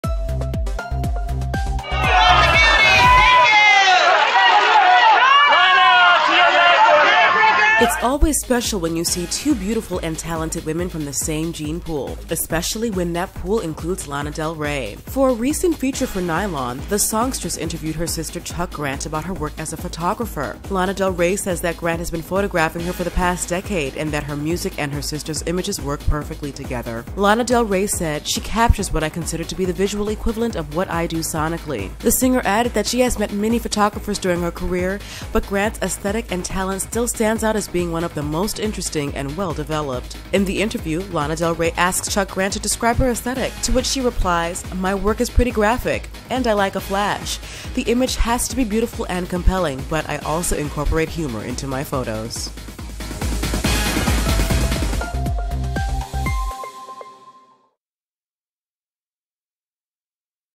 Lana Del Rey Interviews Her Sister Chuck Grant About Photography
It's always special when you see two beautiful and talented women from the same gene pool, especially when that pool includes Lana Del Rey. For a recent feature for NYLON, the songstress interviewed her sister Chuck Grant about her work as a photographer. Lana Del Rey says that Grant has been photographing her for the past decade and that her music and her sister's images work perfectly together.
Lana Del Rey said "She captures what I consider to be the visual equivalent of what I do sonically," . The singer added that she has met many photographers during her career, but Grant's "aesthetic and talent still stands out as being one of the most interesting and well developed.”
In the interview, Lana Del Rey asks Chuck Grant to describe her aesthetic, to which she replies, "My work is pretty graphic, and I like a flash. The image has to be beautiful and compelling, but I also incorporate humor into my photos."